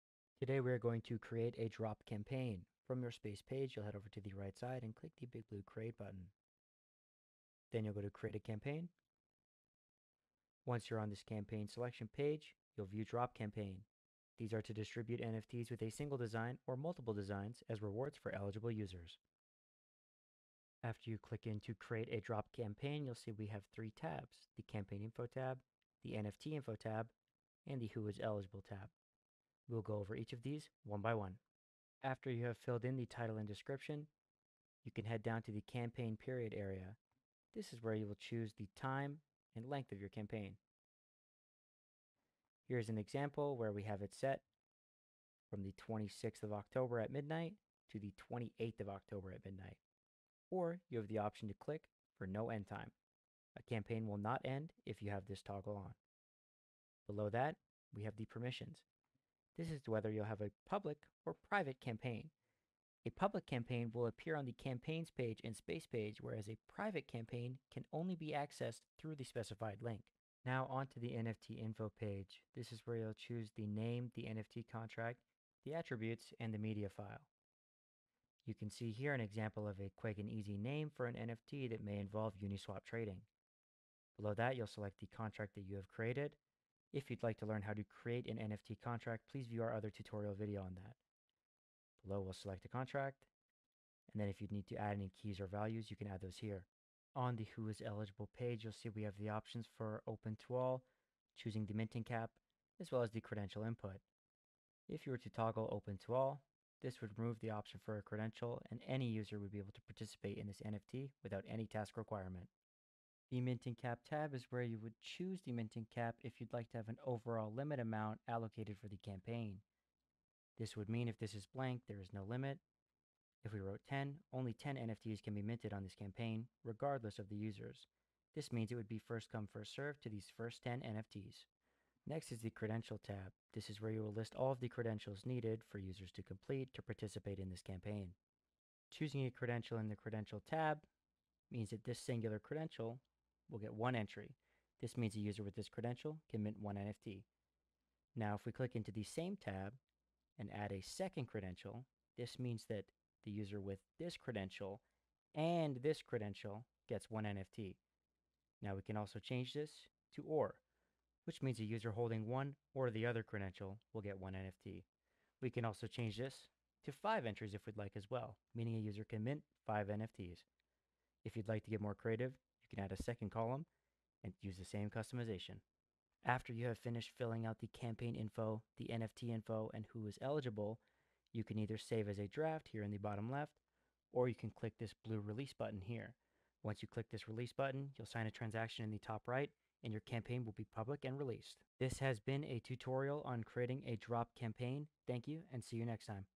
Creating a Drop Campaign on Galxe.
Today we are showing you a walkthrough on how to create a drop campaign using the Galxe dashboard.

Official Links.